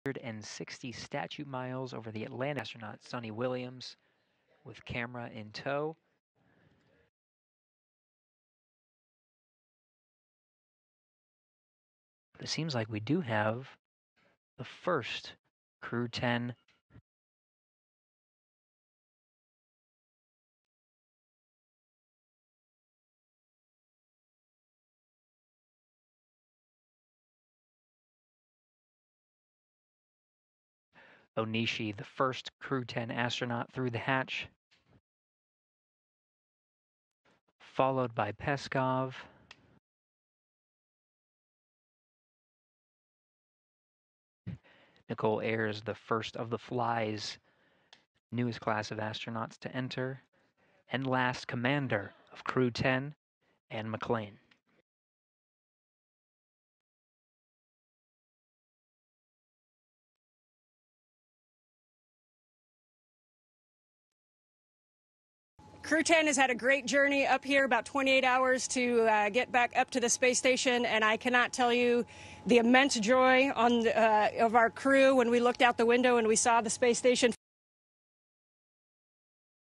NASA Hatch Opening | When Replacements For NASA's Two Stranded Astronauts Arrive At ISS
NASA Space Station | NASA and SpaceX launched a crewed mission to the International Space Station (ISS) to bring home NASA astronauts Sunita Williams and Butch Wilmore, who have been stranded in space since last June.